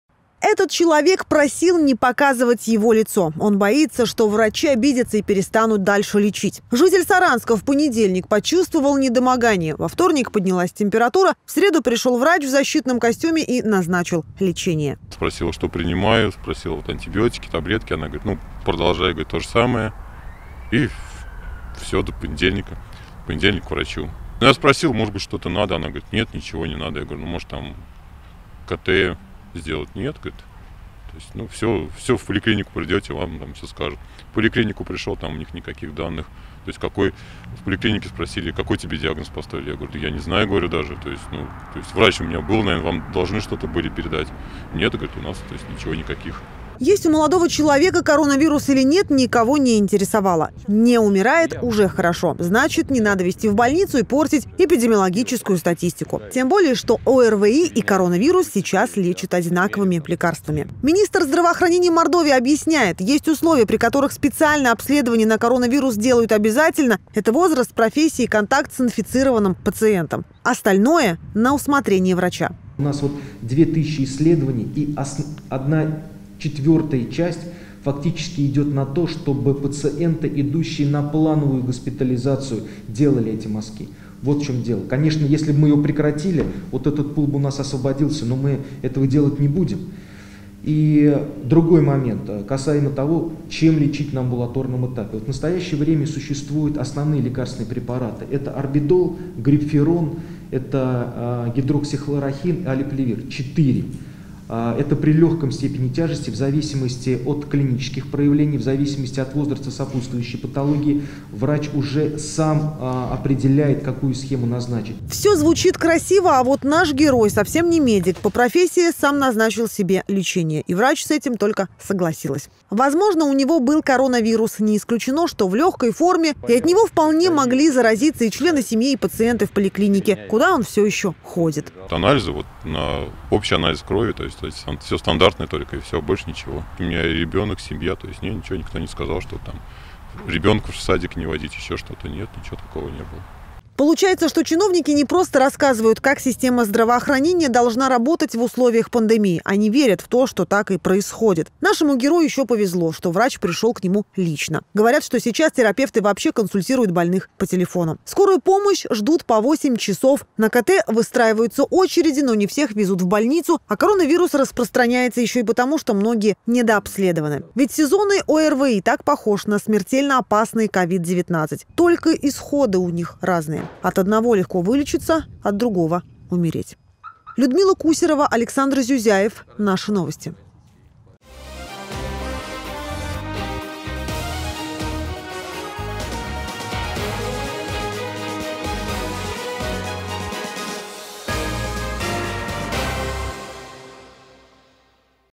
В Саранске врачи назначают лечение на глаз и ставят диагноз ОРВИ
Особенности амбулаторной диагностики. В Саранске врачи назначают лечение на глаз и ставят диагноз ОРВИ. Специальную диагностику для определения возбудителя коронавируса пациенты, которые кашляют и чихают, не проходят. Лечение получают общее для всех вирусных заболеваний. Некоторые после этого поступают в больницу в критическом состоянии.